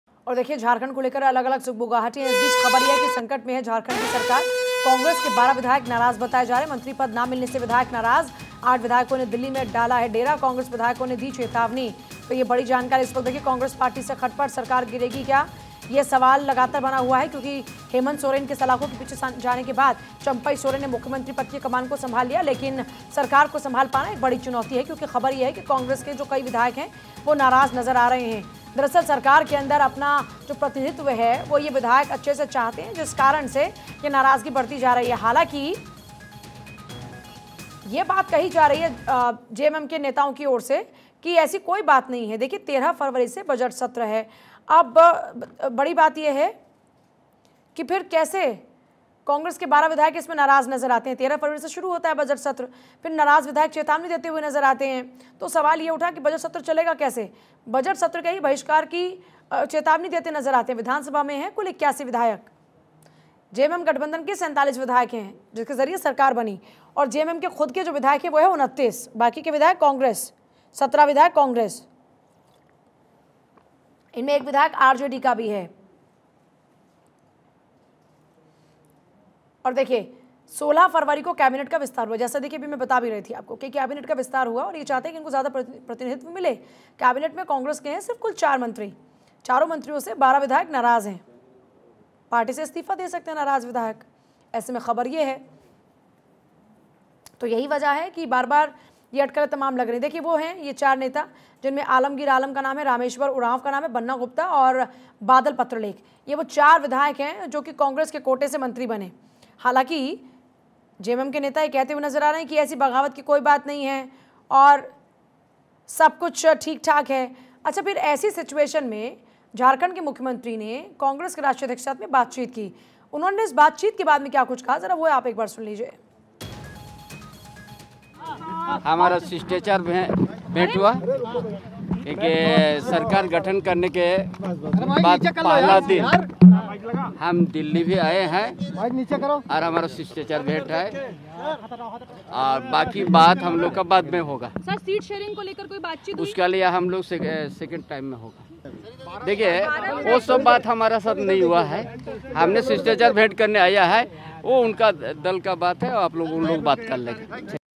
Breaking News : झारखंड में सियासी बग़ावत 16 फरवरी को कैबिनेट विस्तार हुआ | Congress | MLA
झारखंड में सियासी बग़ावत  16 फरवरी को कैबिनेट विस्तार हुआ
कैबिनेट में कांग्रेस के कुल 4 मंत्री चारों मंत्रियों से 12 विधायक नाराज़
पार्टी से इस्तीफा दे सकते हैं नाराज़ विधायक.

Political rebellion in Jharkhand, cabinet expansion on 16 February
A total of 4 Congress ministers in the cabinet, 12 MLAs angry with the four ministers
Angry MLAs may resign from the party.

Anchor Name : Pooja Yadav | Show Name : News | Time : 02: 00 PM | Date :18/02/2024 |